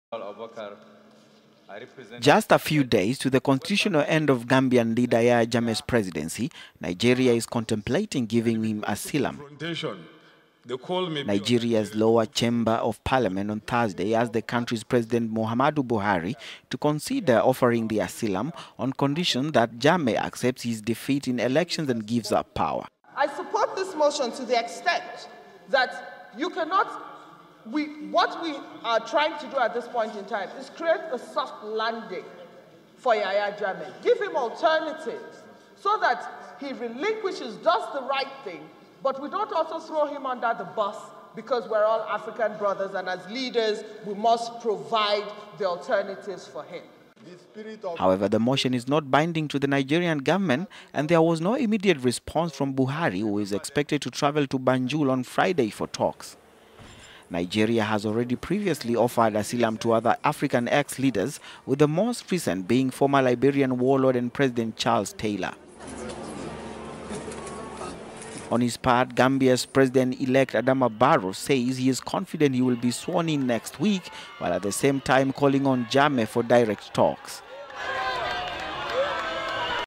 Gambia: Nigerian MPs want asylum for Jammeh
Just a few days to the constitutional end of Gambian leader Yahya Jammeh's presidency, Nigeria is contemplating giving him asylum.

Nigeria's lower chamber of parliament on Thursday asked the country’s President Muhammadu Buhari to consider offering the asylum, on condition that he accepts his defeat in elections and gives up power.

Nnenna Ukeje, of the Foreign Affairs Committee in the House of Representatives said: "I support this motion to the extent that ... what we are trying to do is to cr…